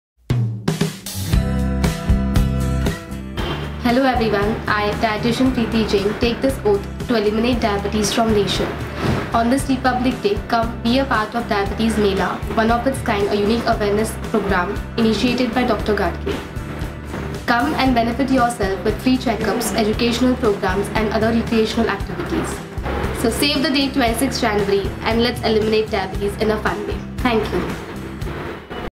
Diabetes Mela - You all are invited
Nutritionist Dr. Preeti Jain invites you all to the most exciting & unique Diabetes Mela you’ve ever attended. She also reveals key highlights of the Mela.